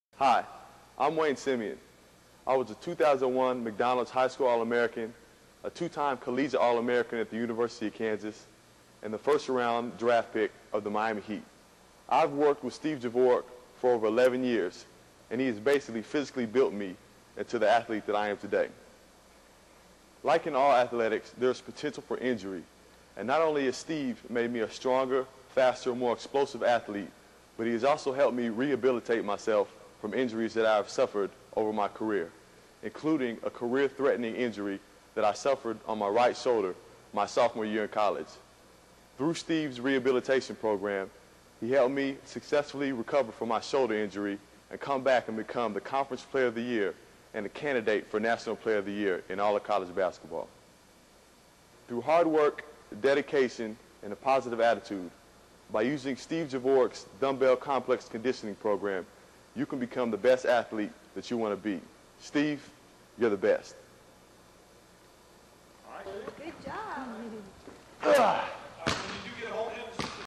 Simien Book Testimonial
Wayne Simien promotes Javorek's Complex Conditioning book